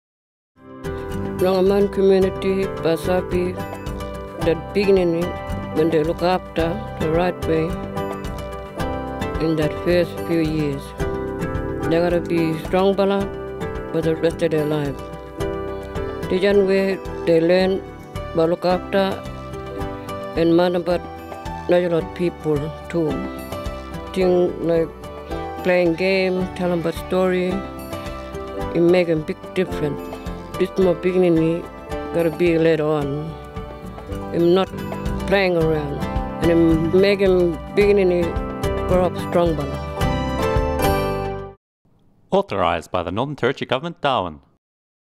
AEDI TV Ad Social Emotional Wellbeing in Katherine Kriol Language
TV ad promoting Social and Emotional Wellbeing for Early Childhood Development Produced for Menzies School of Health and the Australian Early Development index with funds from the NT Government and the Telethon institute.